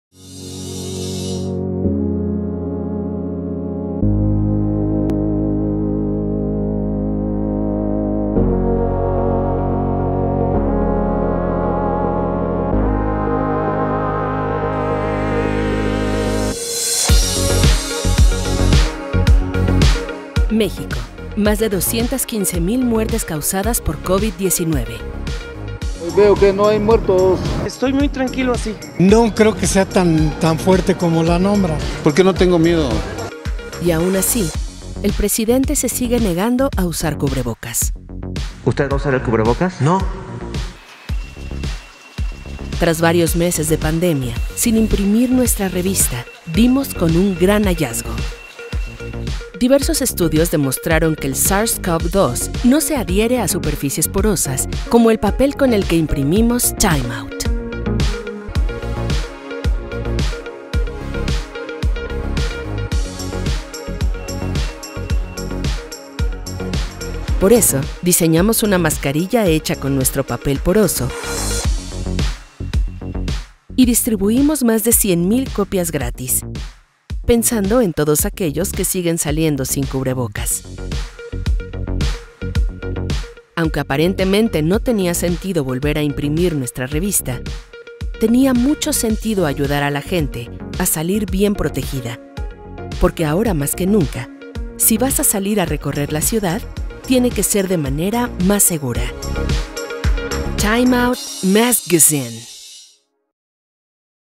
Time Out México- “Maskgazine”:
Grey México y Time Out se aliaron para crear una mascarilla hecha del papel poroso con el que imprime su revista, con la que invitan a salir protegidos a aquellos que siguen sin hacerlo.